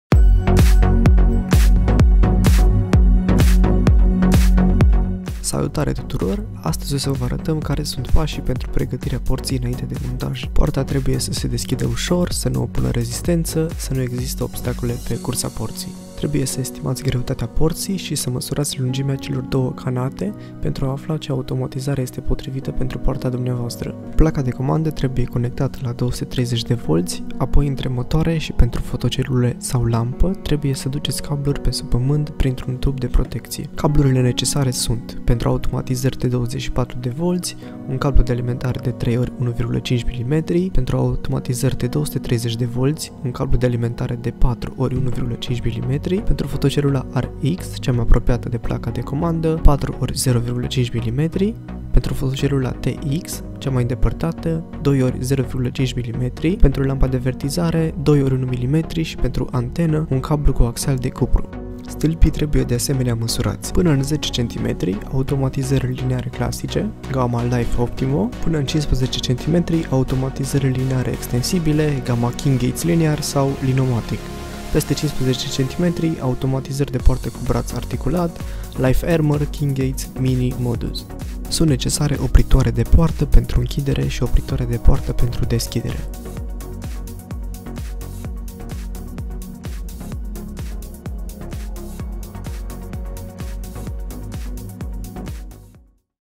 Cum se pregateste poarta inainte de montajul unei automatizari | Automatizari porti
Pasii pentru pregatirea unei porti inainte de montajul unei automatizari.